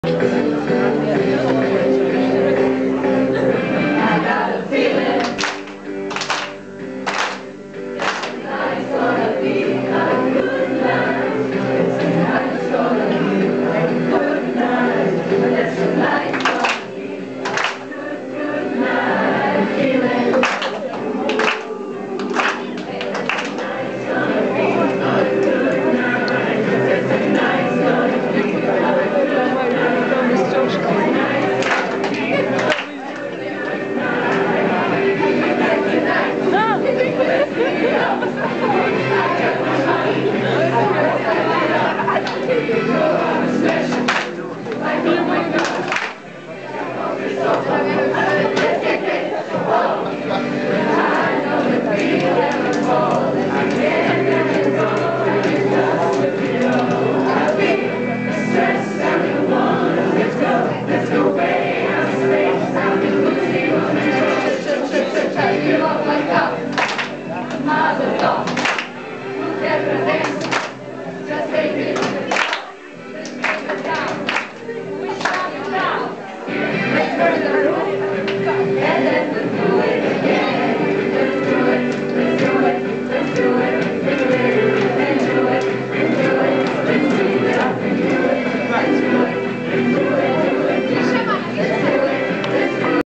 Dan skole Belanovica 3
Osmaci sa svojom nastavnicom muzickog